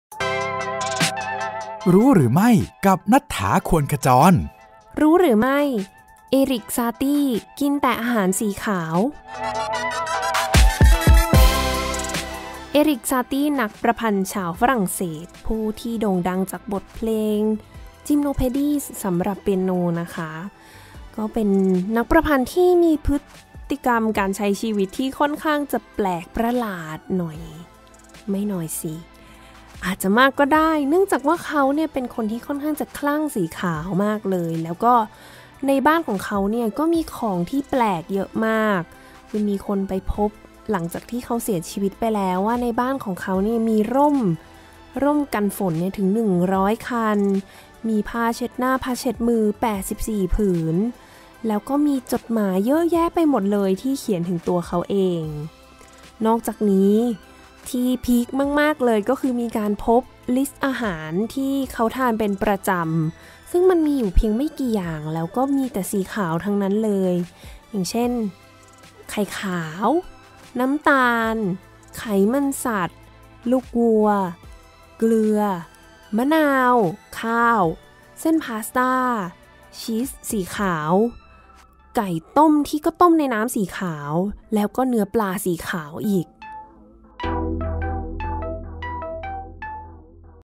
นักประพันธ์ที่กินแต่อาหารสีขาว EP 95 รู้หรือไม่ GenZ and Classical Music
GenZandCalssicalMusic ช่วง รู้หรือไม่ จะนำสาระน่ารู้และเกร็ดต่าง ๆ เกี่ยวกับวงการดนตรีคลาสิก ที่คุณอาจจะไม่เคยรู้มาเล่าสู่กันฟังโดย มุก ณัฏฐา ควรขจร
ฟังรายการได้ทางเว็บไซด์